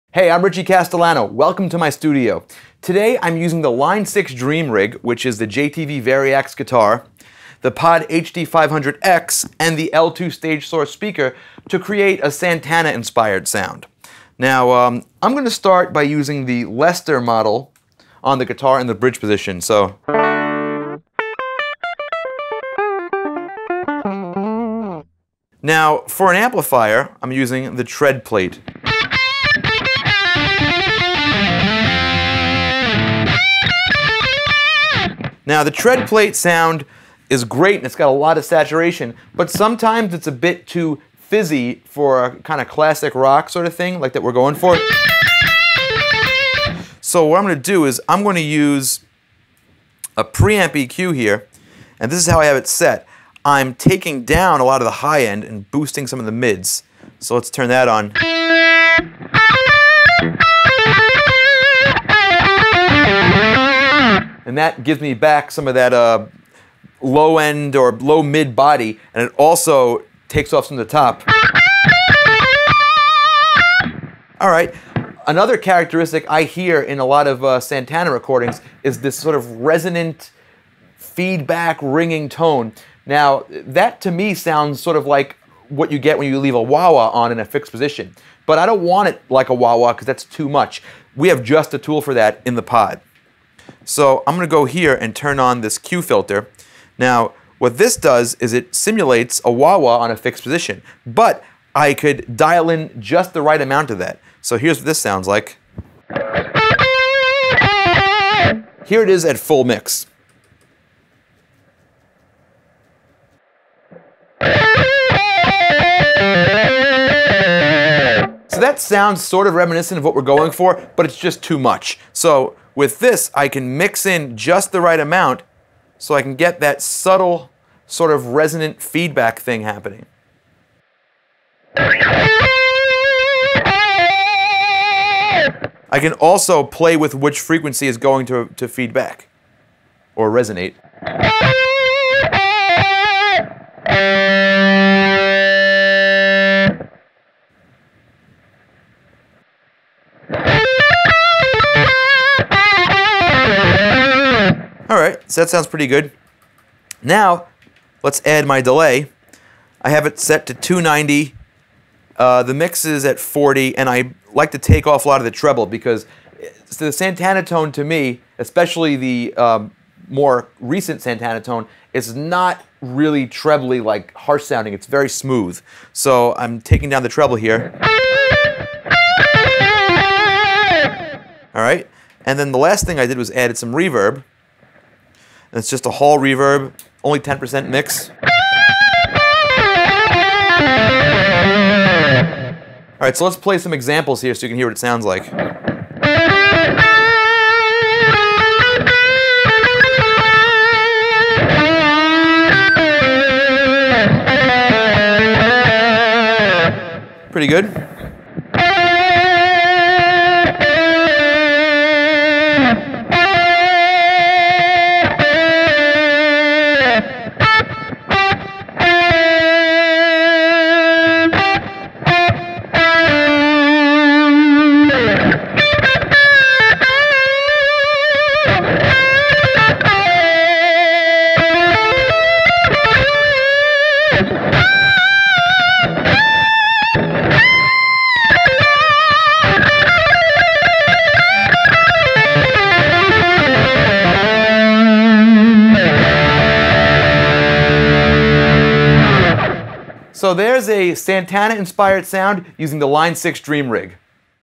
Line 6 Dream Rig - Santana Inspired Tone
In this video, I demonstrate how I create a Santana Inspired lead sound using the Line 6 Pod HD500X, JTV Variax Guitar & L2 StageSource Speaker.